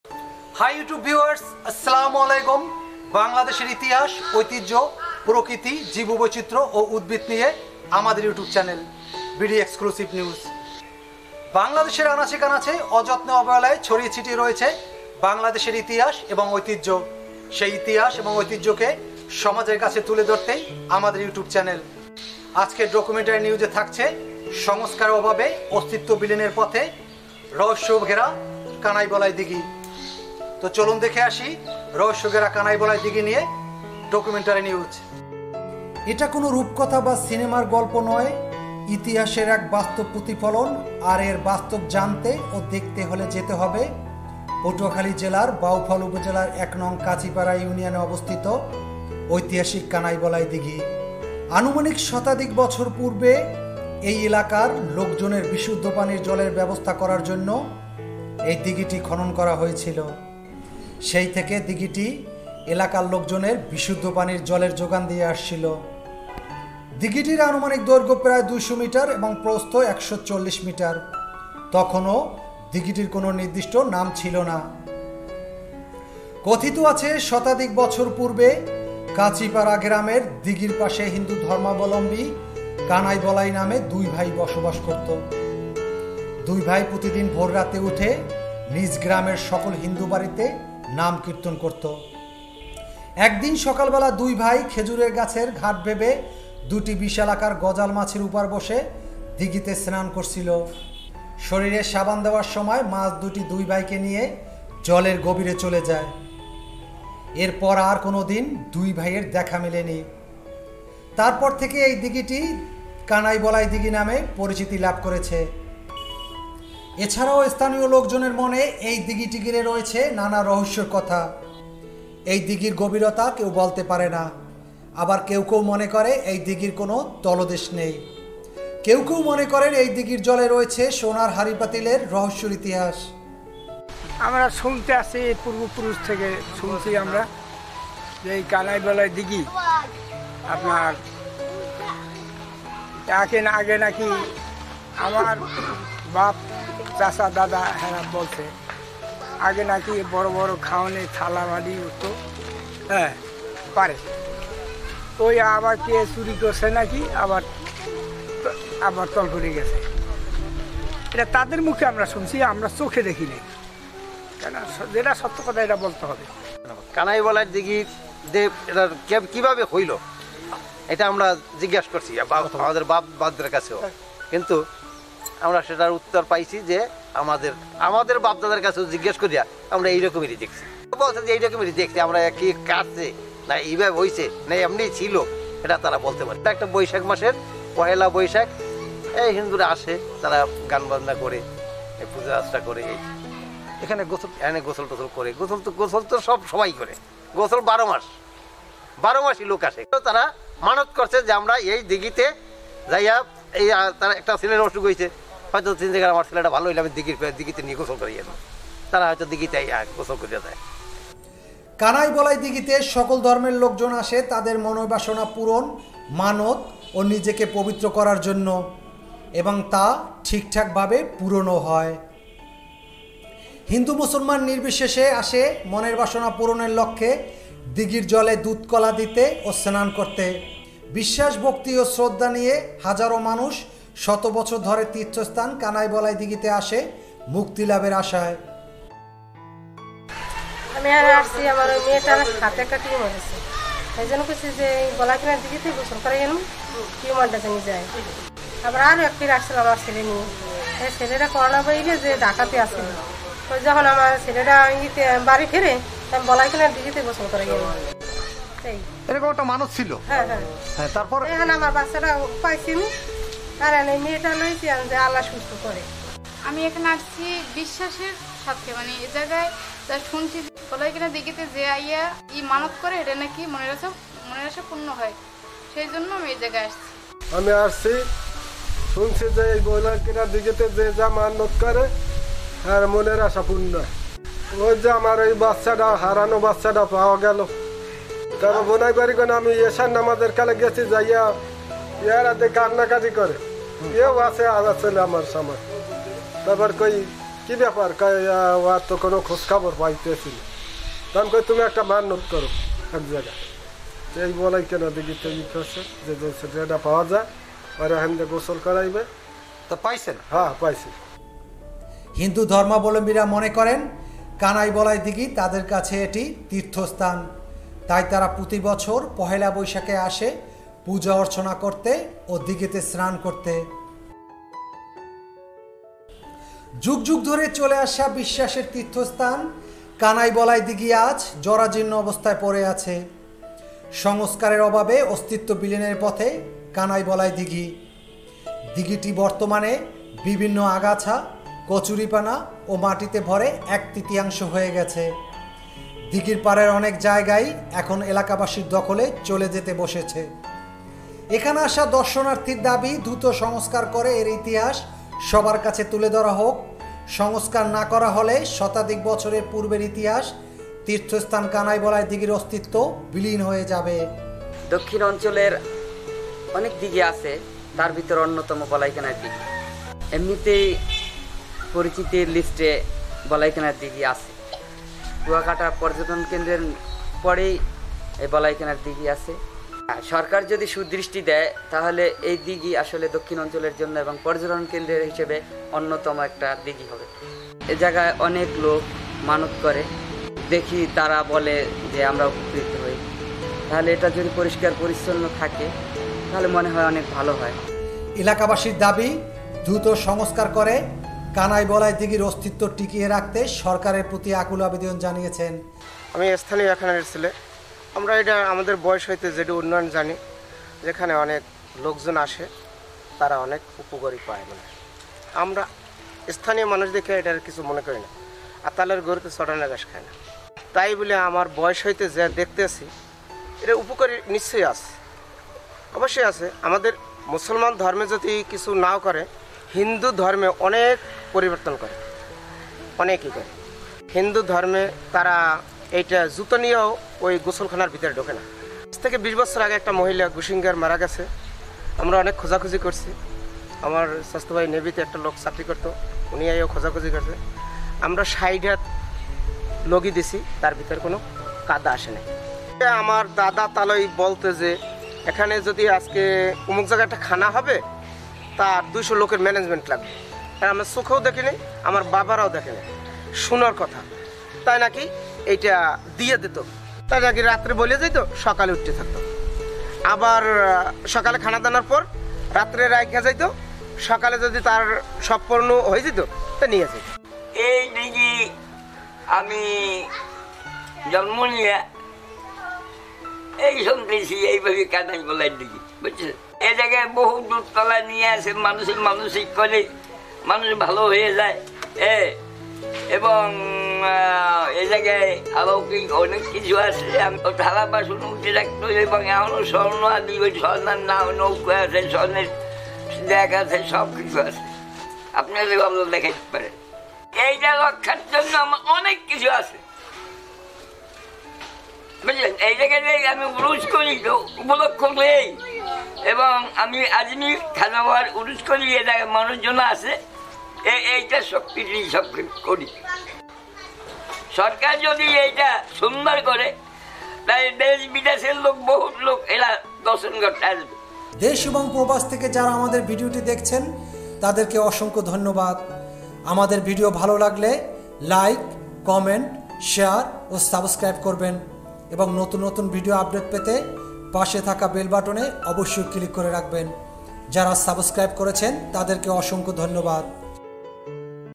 অলৌকিক ভাবে দিঘী থেকে সোনার হাড়ি পাতিল উঠে আসত ! | রহস্যেঘেরা কানাই বলাই দিঘী | BD NEWS HD
এটা কোন রুপকথা বা সিনেমার গল্প নয়, ইতিহাসের এক বাস্তব প্রতিফলন আর এর বাস্তব জানতে ও দেখতে হলে যেতে হবে পটুয়াখালী জেলায় বাউফল উপজেলার ১নং কাছিপাড়া ইউনিয়নে অবস্থিত ঐতিহ্যবাহী কানাই-বলাই দিঘী। আনুমানিক শতাধিক বছর পূর্বে এই এলাকার লোকজনের বিশুদ্ধ পানীয় জলের ব্যবস্থা করার জন্য এই দিঘীটি খনন করা হয়েছিল। সেই থেকে দিঘীটি এলাকার লোকজনের বিশুদ্ধ পানীয় জলের যোগান দিয়ে আসছিল।দিঘীটির আনুমানিক দৈর্ঘ্য প্রায় দুইশ’ মিটার ও প্রস্থ একশ’ চল্লিশ মিটার। তখনও দিঘীটির কোন নির্দিষ্ট নাম ছিলনা।
কথিত আছে, শতাধিক বছর পূর্বে কাছিপাড়া গ্রামের দিঘীর পাশে হিন্দু ধর্মাবলম্বী কানাই-বলাই নামে দুই ভাই বসবাস করত। দুই ভাই প্রতিদিন ভোর রাতে উঠে নিজ গ্রামের সকল হিন্দু বাড়িতে নাম কীর্তন করতো।একদিন সকালবেলা দুই ভাই খেজুর গাছের ঘাট ভেবে দুটি বিশালাকার গজাল মাছের উপর বসে দিঘীতে স্নান করছিল, শরীরে সাবান দেয়ার সময় মাছ দুটি দুই ভাইকে নিয়ে জলের গভীরে চলে যায়। এরপর আর কোনোদিন দুই ভাইয়ের দেখা মেলেনি। তারপর থেকেই এ দিঘীটি কানাই-বলাই দিঘী নামে পরিচিতি লাভ করেছে।
এছাড়াও স্থানীয় লোকজন মনে এই দিঘী ঘিরে রয়েছে নানান রহস্যজনক কথা। এই দিঘীর গভীরতা কেউ বলতে পারেন না।আবার কেউ মনে করেন এই দিঘীর কোন তলদেশ নেই। কেউ কেউ মনে করেন এই দিঘীর জলে রয়েছে সোনার হাড়ি পাতিলের রহস্যের ইতিহাস।
হিন্দু ধর্মাবলম্বীরা মনেকরেন কানাই-বলাই দিঘী তাদের কাছে এটি তীর্থস্থান।তাই তারা প্রতি বছর পহেলা বৈশাখে আসে পূজাঅর্চনা করতে ও দিঘীতে স্নান করতে।
কানাই-বলাই দিঘীতে সকল ধর্মের লোকজন আসে তাদের মনবাসনা পূরণ, মানত ও নিজেকে পবিত্র করার জন্য এবং তা ঠিকঠাক ভাবেই পূরণ হয়।
হিন্দু,মুসলমান নির্বিশেষে আসে মনবাসনা পূরণের লক্ষ্যে, দিঘীর জলে দুধ, কলা দিতে ও স্নান করতে। বিশ্বাস, ভক্তি ও শ্রদ্ধা নিয়ে হাজারও মানুষ শত বছর ধরে তীর্থস্থান কানাই-বলাই দিঘীতে আসে মুক্তিলাভের আশায়।
যুগযুগ ধরে চলে আসা বিশ্বাসের তীর্থস্থান কানাই-বলাই দিঘীটি আজ জরাজীর্ণ অবস্থায় পড়ে আছে। সংস্কারের অভাবে অস্তিত্ব বিলীনের পথে কানাই-বলাই দিঘী। দিঘীটির বর্তমানে বিভিন্ন আগাছা, কচুরিপানা ও মাটিতে ভরে এক তৃতীয়াংশ হয়ে গেছে।দিঘীর পাড়ের অনেক জায়গাই এখন এলাকাবাসীর দখলে চলে যেতে বসেছে।
এখানে আসা দর্শনার্থীদের দাবী দ্রত সংস্কার করে এর ইতিহাস সবার কাছে তুলে ধরা হোক। সংস্কার না করা হলে শতাধিক বছর পূর্বের ইতিহাস তীর্থস্থান কানাই-বলাই দিঘীর অস্তিত্ব বিলীন হয়ে যাবে।
এলাকাবাসির দাবী ধ্রুত সংস্কার করে কানাই-বলাই দিঘীর অস্তিত্ব টিকিয়ে রাখতে সরকারের প্রতি আকুল আবেদন জানিয়েছেন।